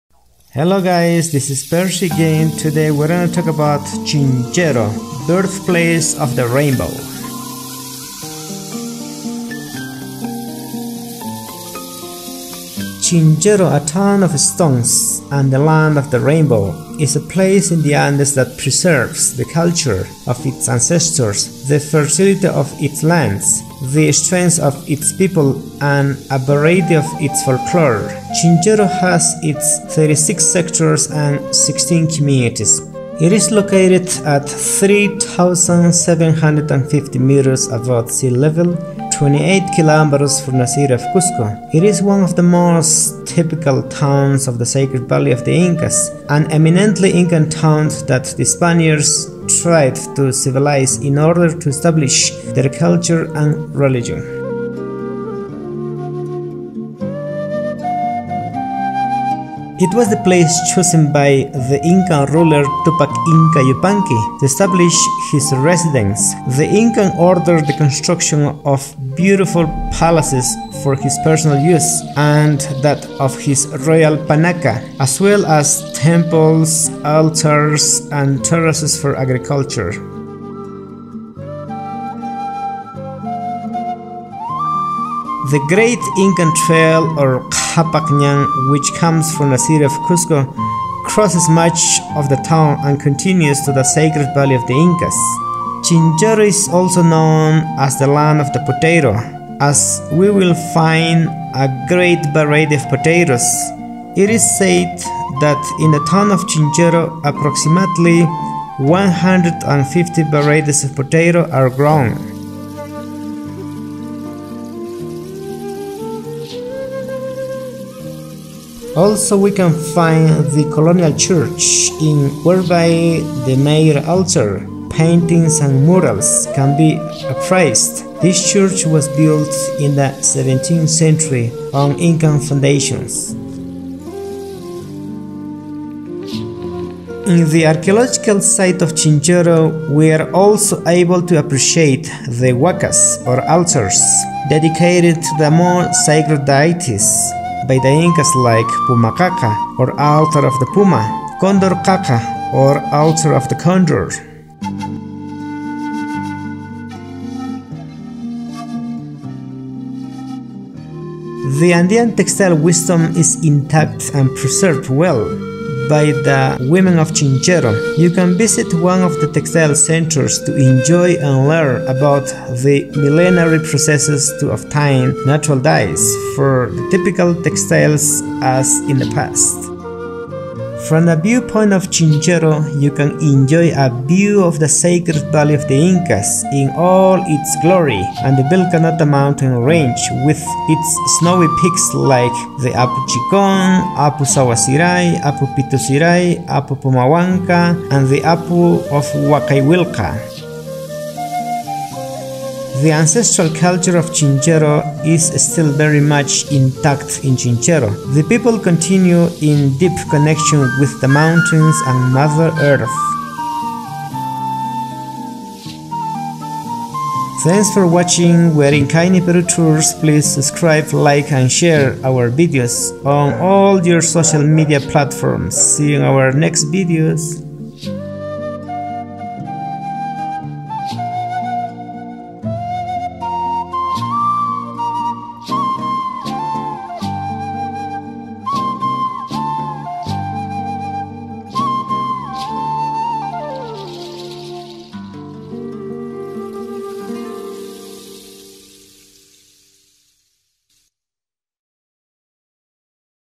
CHINCHERO | Birthplace of the Rainbow
Chinchero, a town of stone and the land of the rainbow, is a place in the Andes that preserves the culture of its ancestors, the fertility of its lands, the strength of its people and a variety of its folklore. Chinchero has its 36 sectors and 16 communities.
It is located at 3750 meters above sea level, 28 km from the city of Cusco. It is one of the most typical towns of the Sacred Valley of the Incas. An eminently Inca town that the Spaniards tried to civilize in order to establish their culture and religion
It was the place chosen by the Inca ruler Túpac Inca Yupanqui to establish his residence. The Inca ordered the construction of beautiful palaces for his personal use and that of his royal panaca, as well as temples, altars and terraces for agriculture.
The great Inca road or Qapaq Ñan, which comes from the city of Cusco, crosses much of the town and continues to the sacred valley of the Incas.
Chinchero is also known as the land of the potato as we will find a great variety of potatoes. It is said that in the town of Chinchero approximately 150 varieties of potatoes are grown.
Also we can find the colonial church in whereby the major altar,  paintings and murals can be appraised. This church was built in the 17th century on Inca foundations.
In the archaeological site of chinchero,  we are also able to appreciate the wakas or altars dedicated to the most sacred deities by the Incas like Pumaqaqa or altar of the puma, Condor Qaqa or altar of the condor.
The Andean textile wisdom is intact and preserved well by the women of Chinchero. You can visit one of the textile centers to enjoy and learn about the millenary processes to obtain natural dyes for the typical textiles as in the past.
From the viewpoint of Chinchero you can enjoy a view of the sacred valley of the Incas in all its glory and the Vilcanota mountain range with its snowy peaks like the apu Chicon, apu Sawasiray, apu Pitusiray apu Pumahuanca and the apu Wakay willka.